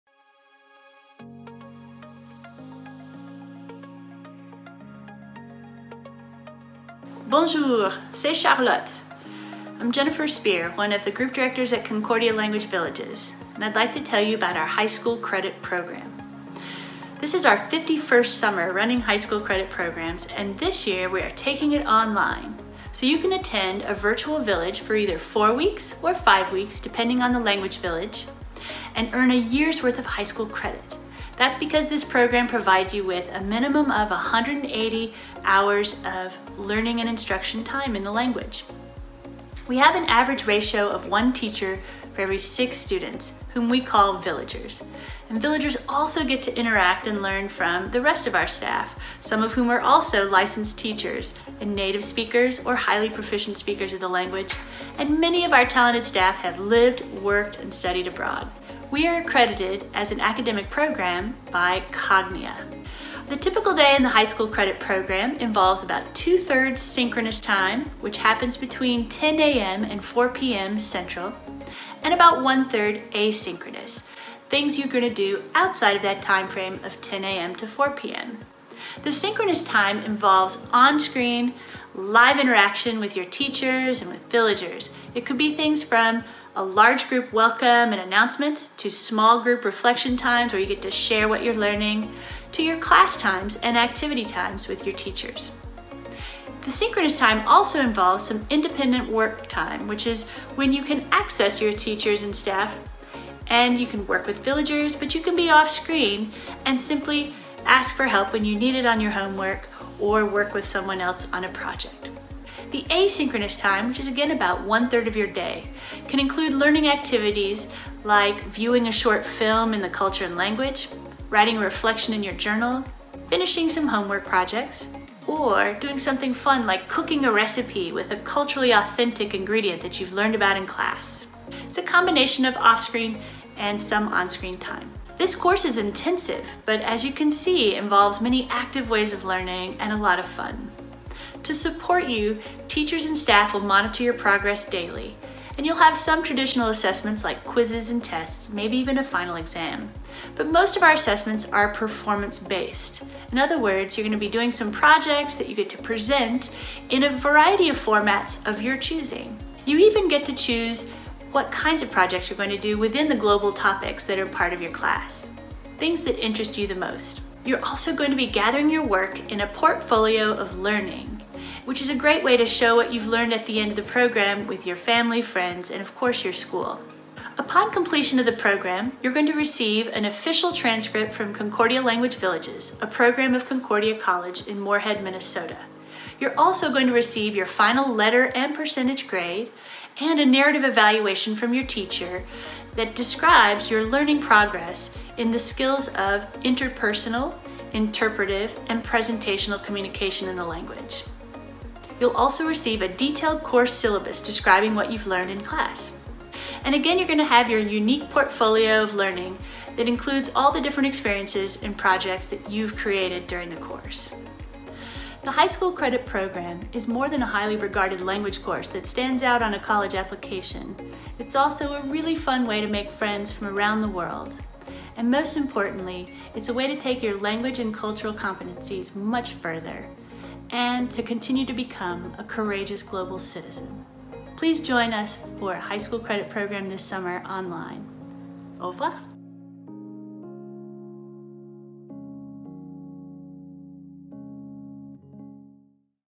High School Credit Virtual Villages Program
Charlotte is one of three Group Directors at Concordia Language Villages. Here, she explains the value our High School Credit Virtual Villages Program has to offer villagers! Not only is it a fun way to learn, but villagers can also earn a full year of high school credit. Both four- and five-week options are available.